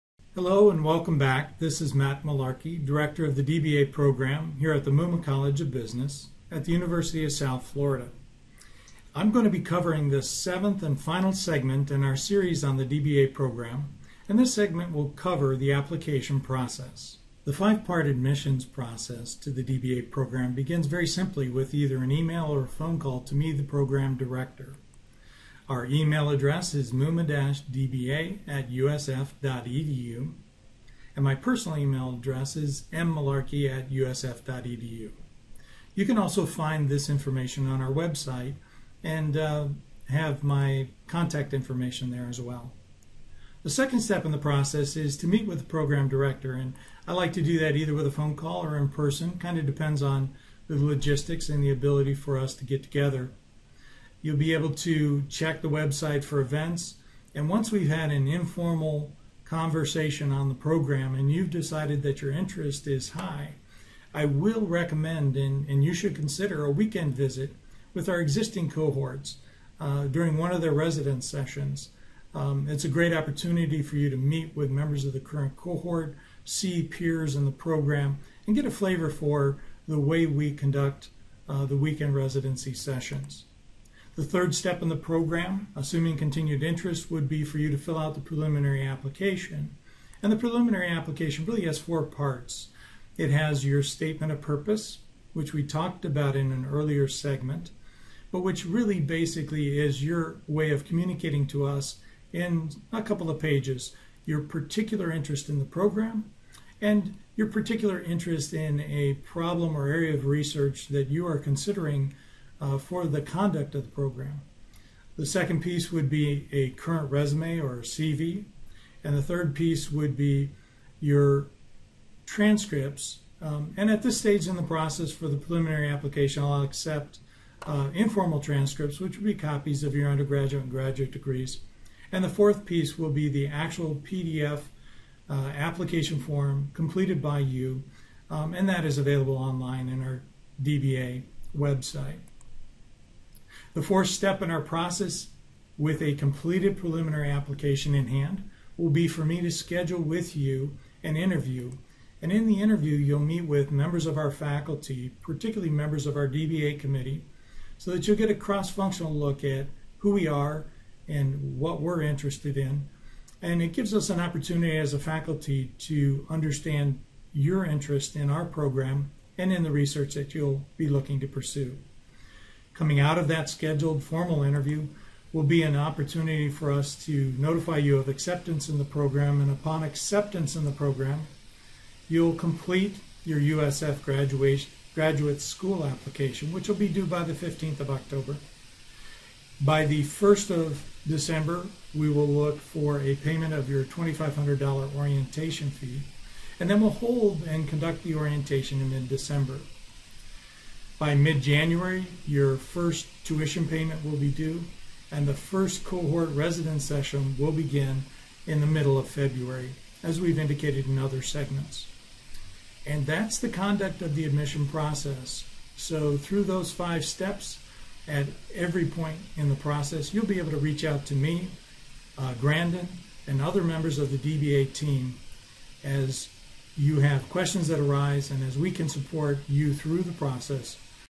USF DBA 2016 Video 7: Application Process
Information on the Muma College of Business at USF Doctor of Business Administration program, particularly aimed at individuals seeking admission for the cohort starting in February 2016.